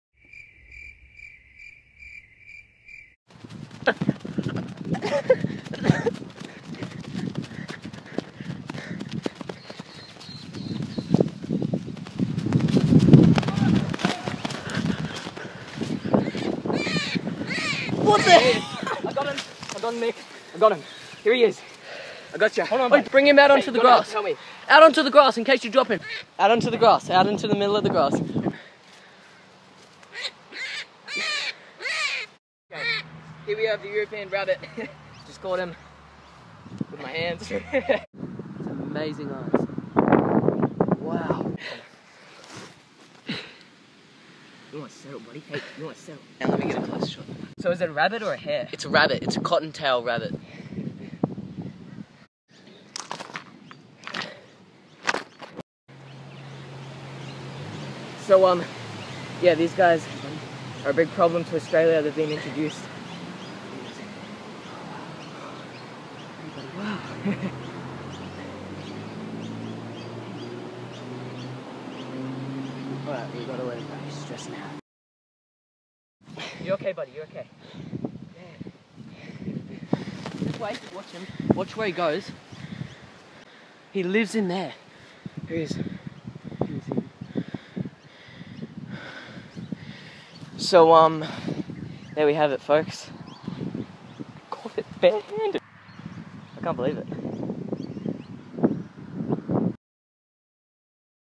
[MeredithWolfe] Hare chased and caught BAREHANDED | TDB
[MeredithWolfe] After going for a walk we noticed a big brown hare grazing on an open field. Well what happens next is unbelievable... We chase down and manage to catch the hare with our barehands! Simple as that. No traps, no guns, no fuss. (We admit we were pretty surprised catching it though!)